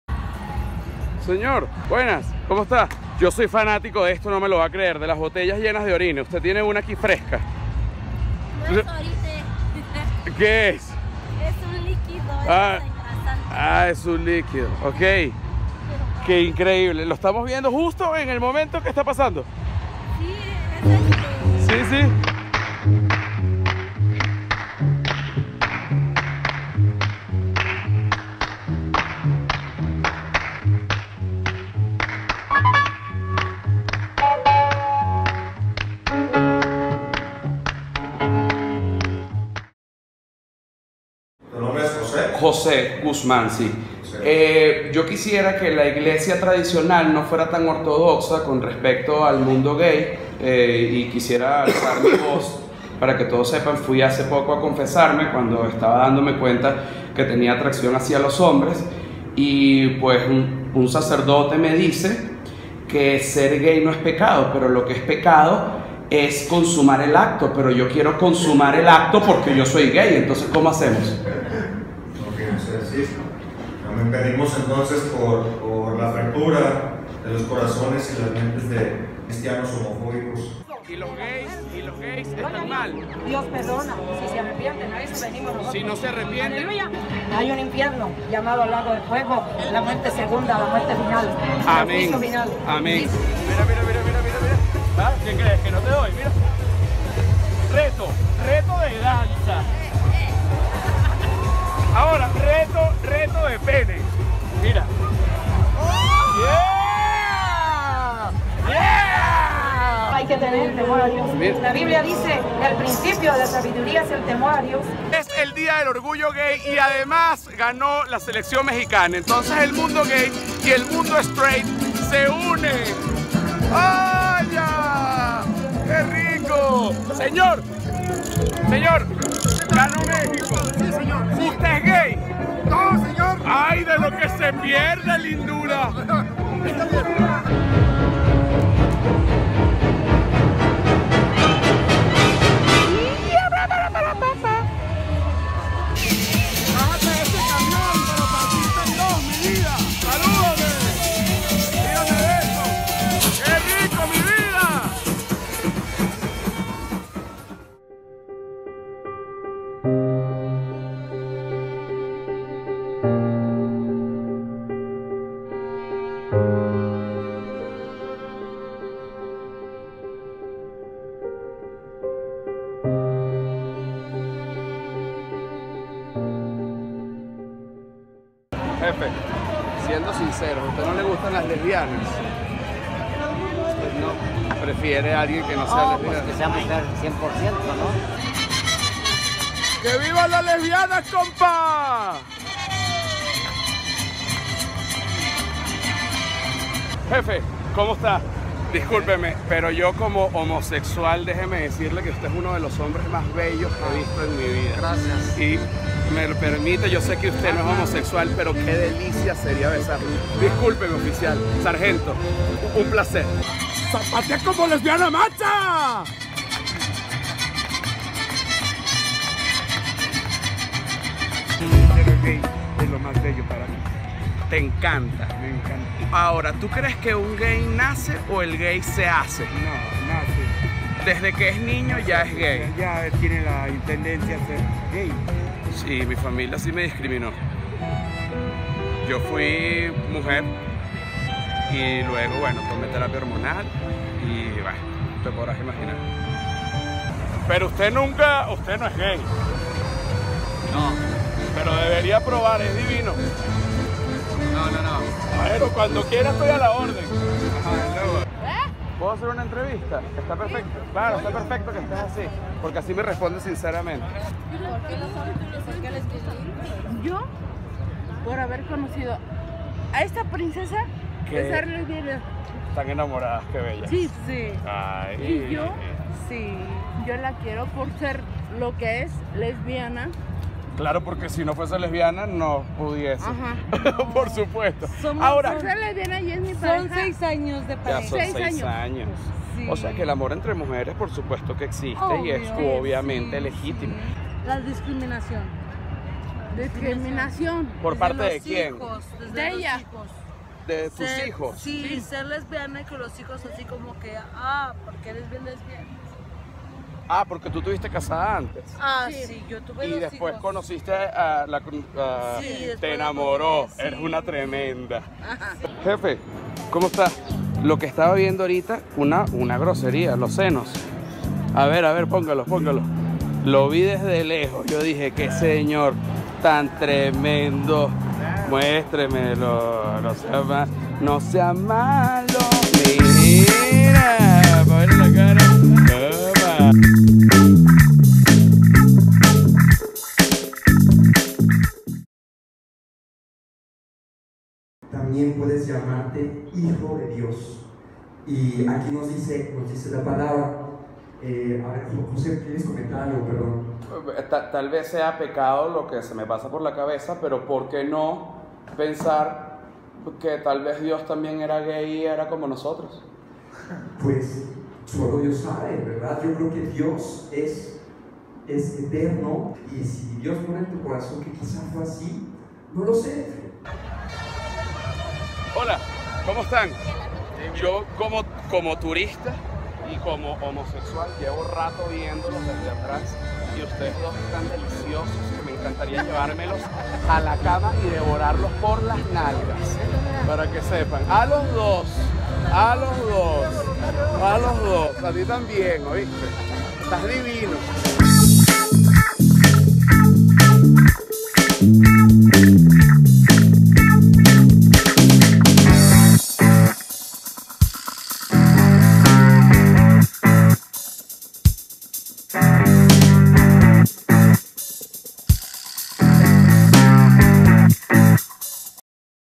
Así es el día del orgullo en Ciudad de México.
(Lugar que me llena de orgullo y quiero que visiten)